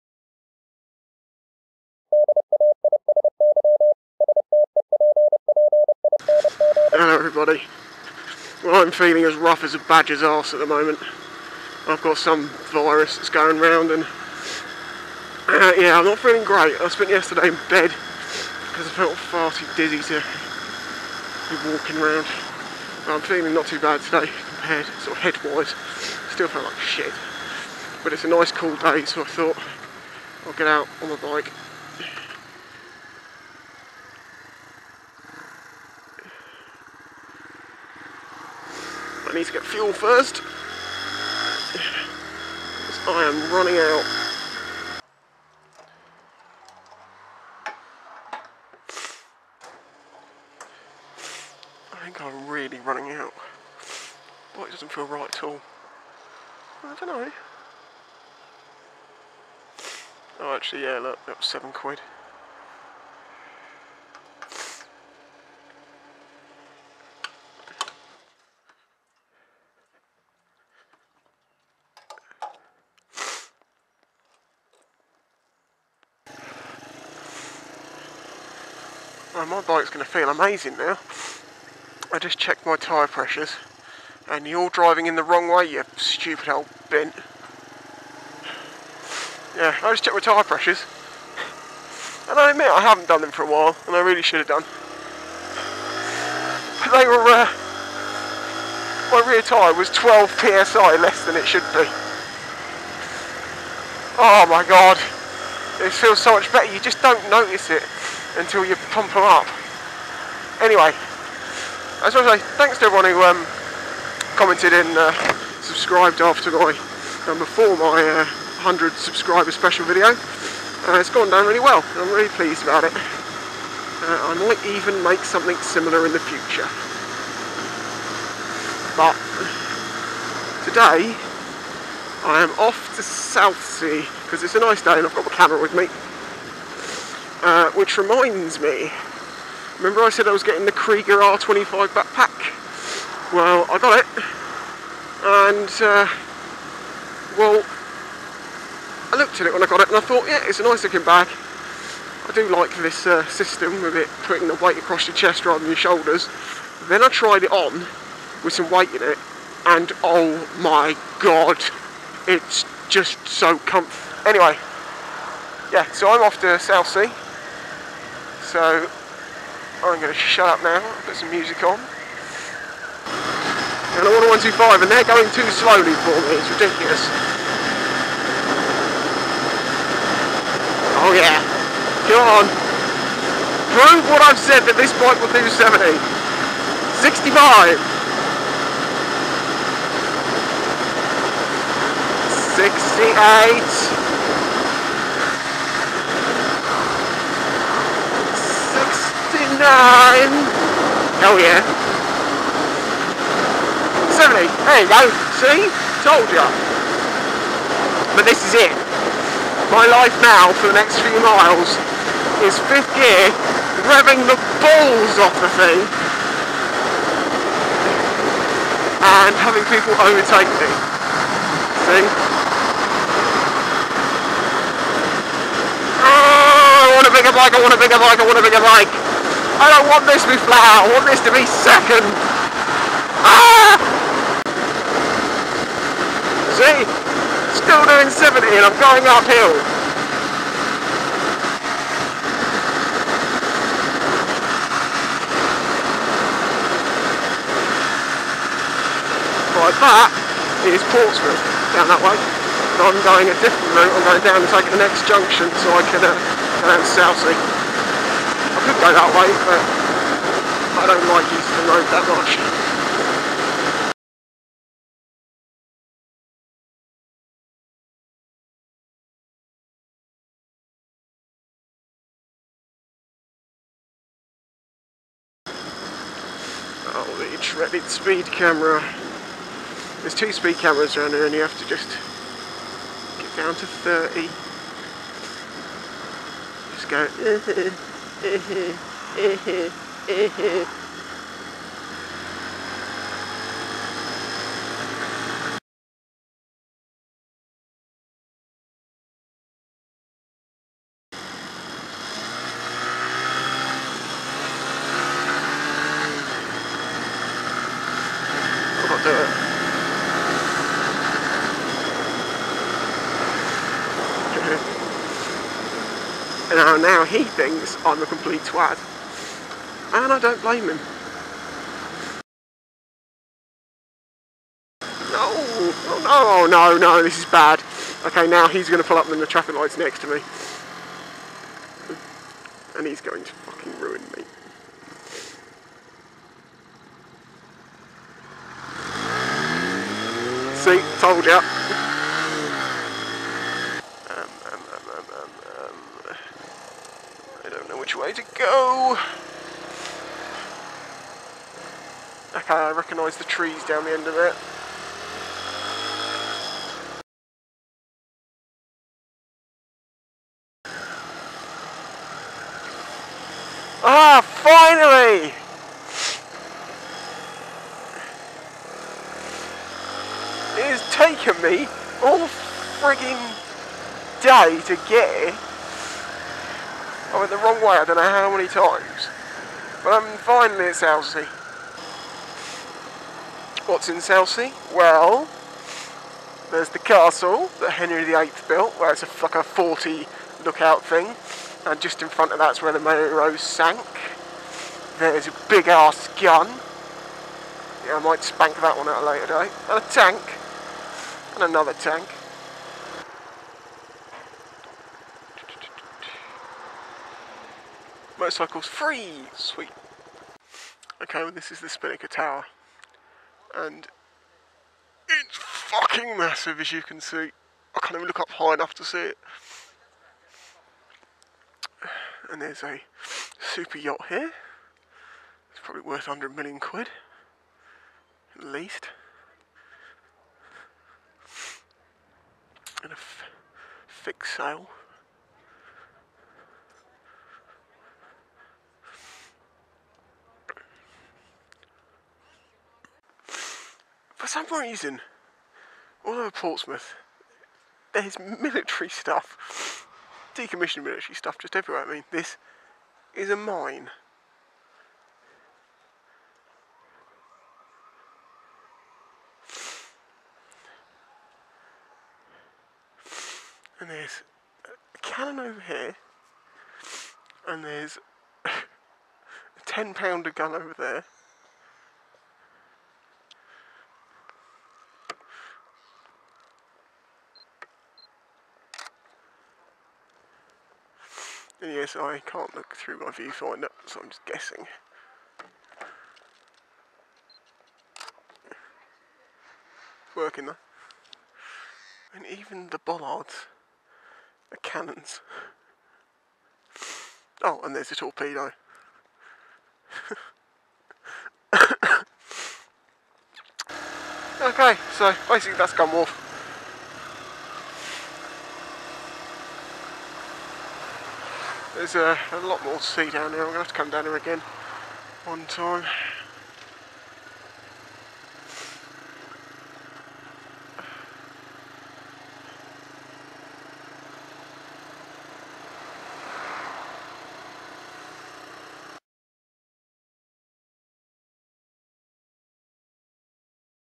Guns everywhere!!
Hi all

I have a virus at the moment so went for a ride to get some fresh air!

Went to southsea where there are guns everywhere! _____
Want a action camera like mine?

The code will not work on items already under a offer, But it will with some so worth a try! .